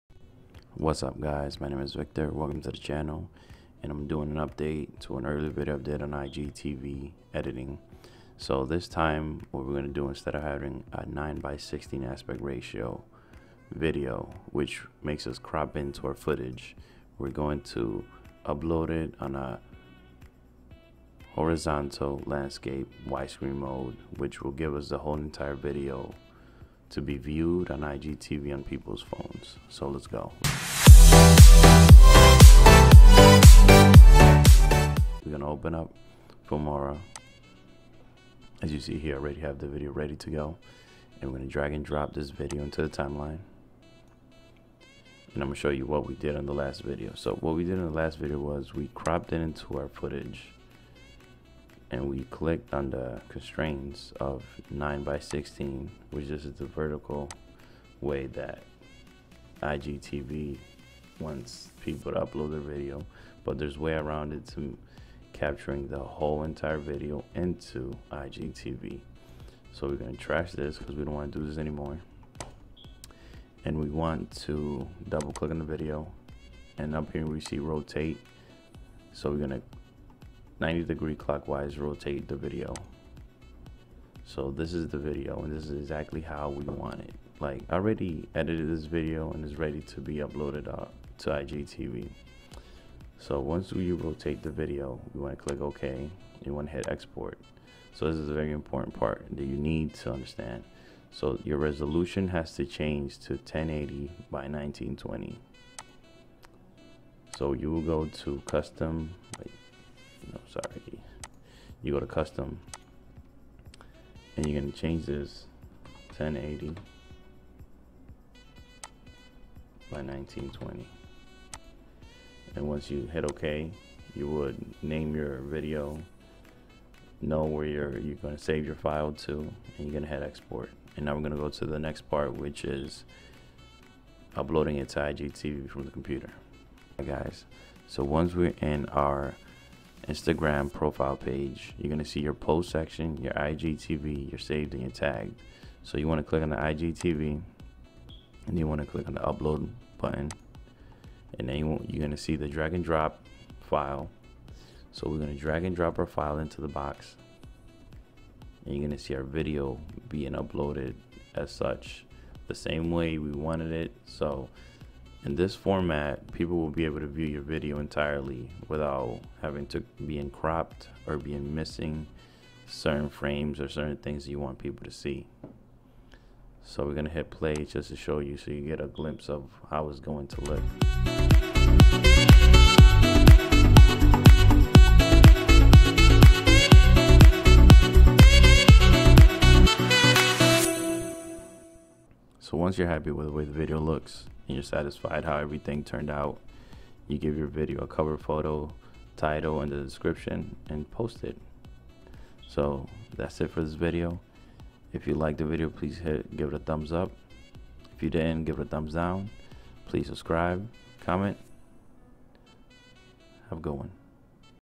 How to Upload Horizontal, Widescreen, Landscape Videos on IGTV? (Tutorial)
If you were looking to upload your video to IGTV on a landscape, horizontal, or widescreen this will show you on how to do it with Filmora Wondeshare a free software for editing videos. I will take you on an easy route just to do that.

 Artist: Ehrling
 Download/Stream: /yOMdzzl2

 Artist: Vexento
 Download/Stream: /K6DD2KMr